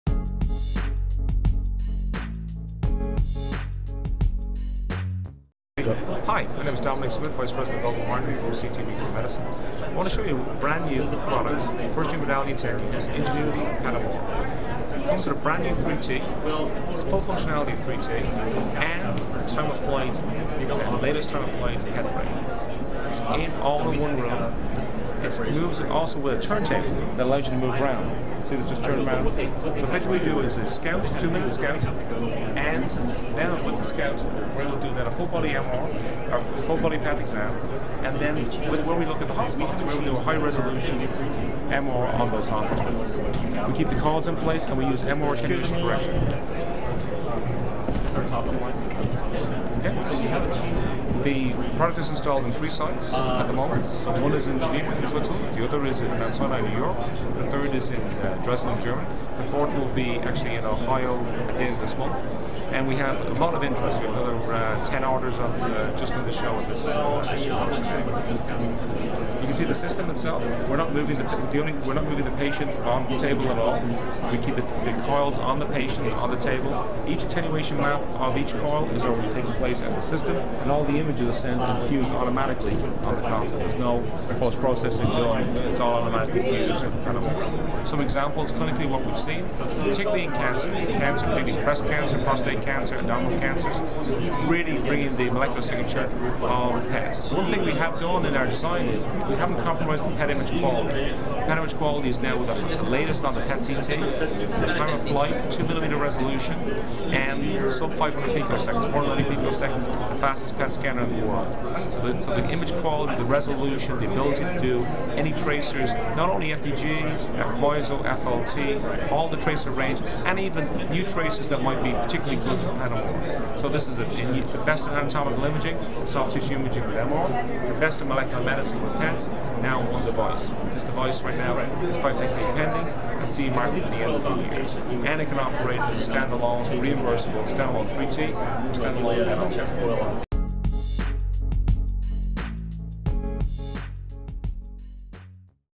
Philips Unveils Ingenuity TF PET/MR Combo
At RSNA 2010, Philips unveiled a 3T MR and a high resolution PET scanner with an integrated rotating table that passes the patient from one machine immediately into the other. Medgadget asked the folks at Philips to explain the new system and the advantages of keeping the two scanning modalities separate